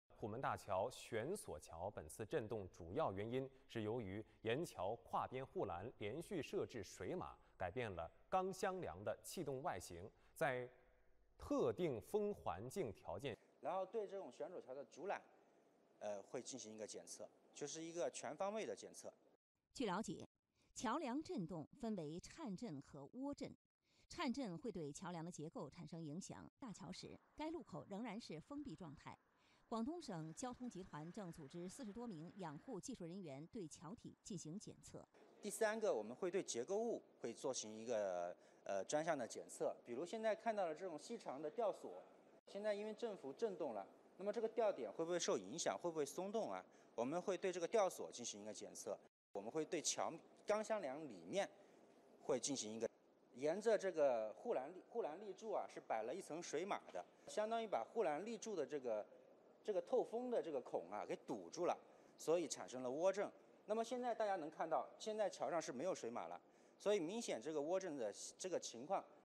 Cina, il ponte sembra di gomma: la polizia decide la chiusura per evitare rischi
Immagini incredibili arrivano dalla provincia di Guangdong, in Cina, dove il ponte Humen - che collega la città di Guangzhou a Dongguan - ha iniziato a tremare vistosamente. Secondo i media locali, la causa delle oscillazioni sarebbero insoilte raffiche di vento. Per evitare qualsiasi tipo di rischio, le autorità hanno deciso la chiusura. I tecnici sono a lavoro giorno e notte per riparare la struttura. .  .Il ponte, che è stato costruito nel 1992, ha aperto al transito dei veicoli a partire dal 1997.